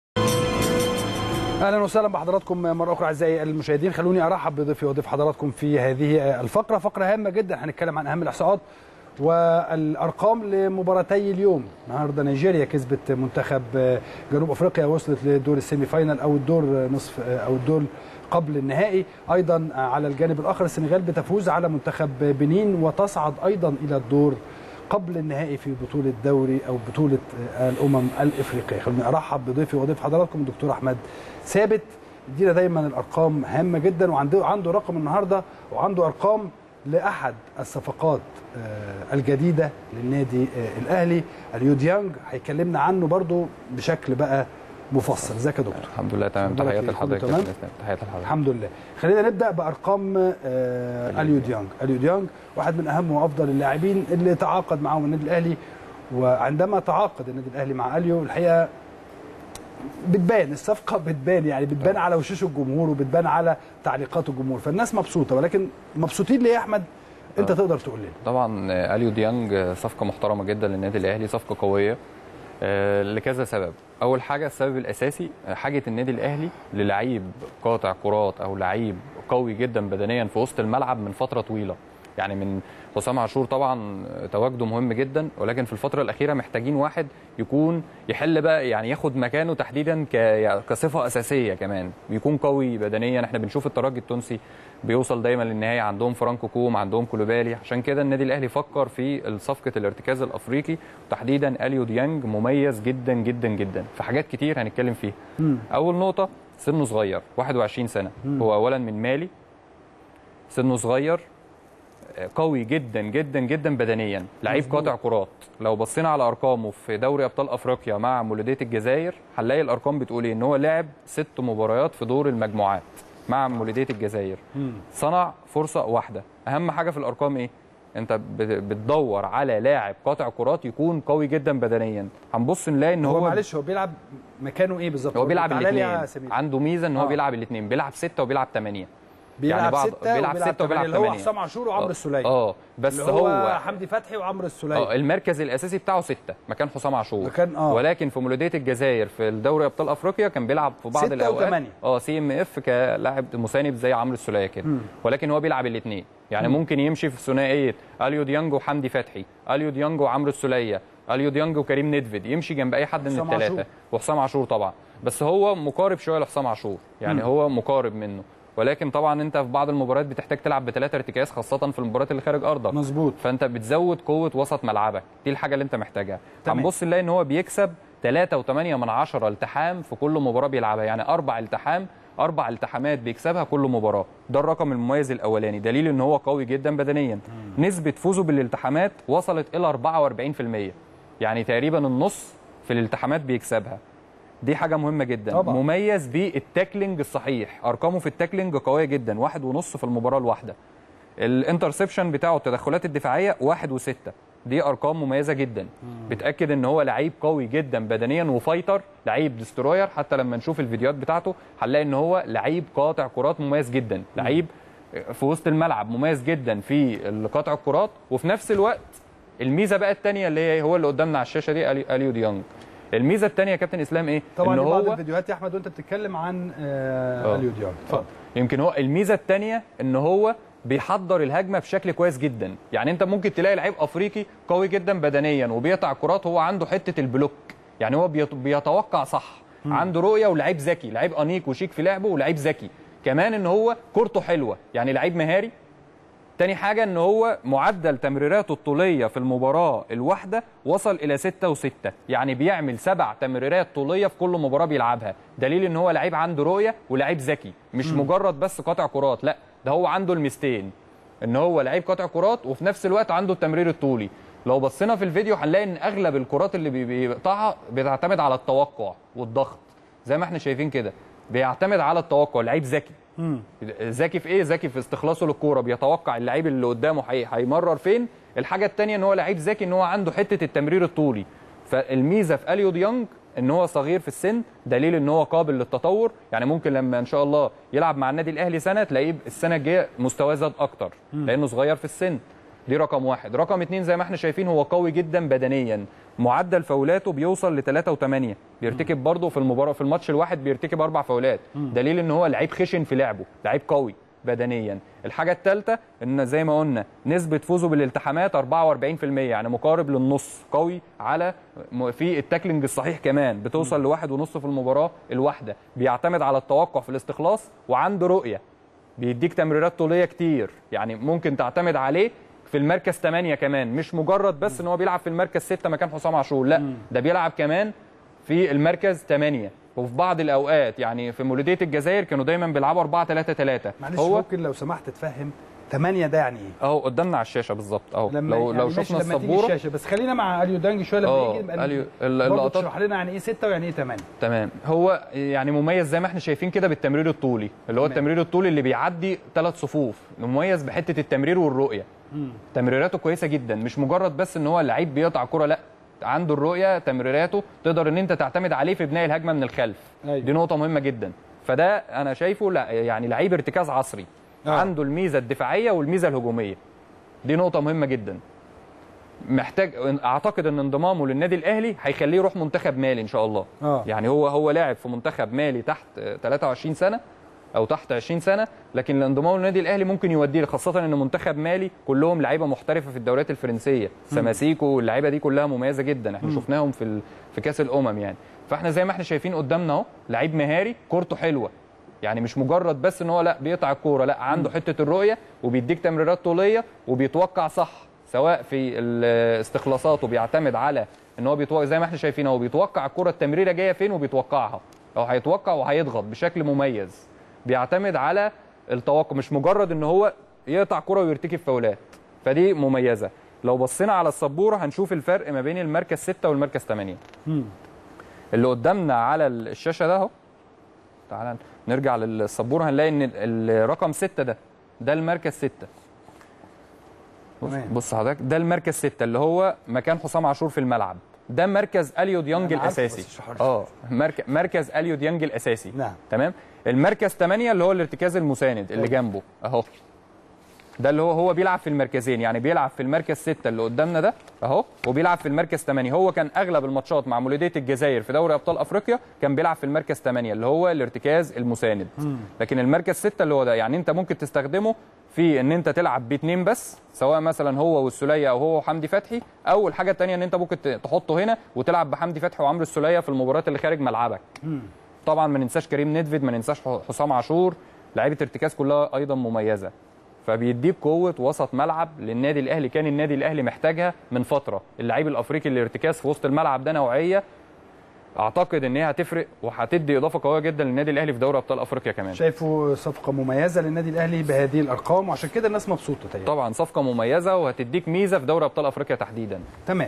شاهد "كشف حساب" من احمد ثابت عن مميزات صفقة الاهلى الجديده "أليو ديانج"
التردد: 12341 أفقي.
القمر: نايل سات.
معامل الخطأ: 5/6.
معدل الترميز: 27500